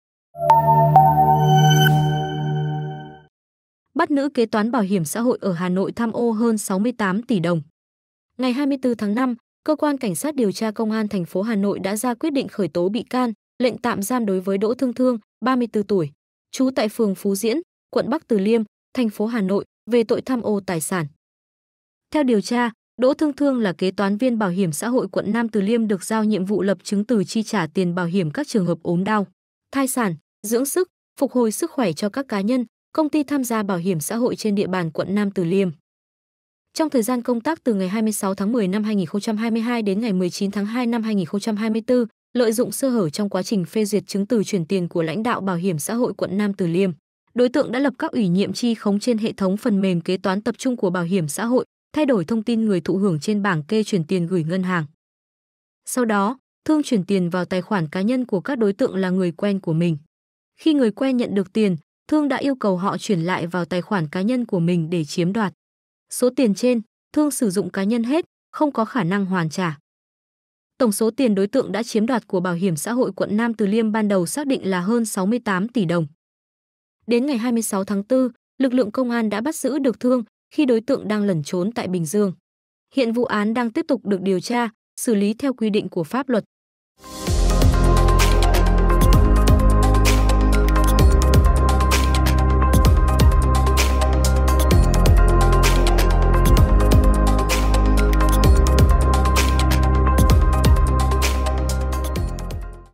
Bắt nữ kế toán BHXH ở Hà Nội tham ô hơn 68 tỉ đồng I Viettimes
Lợi dụng sơ hở của cơ quan, nữ kế toán Bảo hiểm xã hội (BHXH) Q.Nam Từ Liêm (Hà Nội) đã lập khống các ủy nhiệm chi, thay đổi thông tin người thụ hưởng và chiếm đoạt hơn 68 tỉ đồng.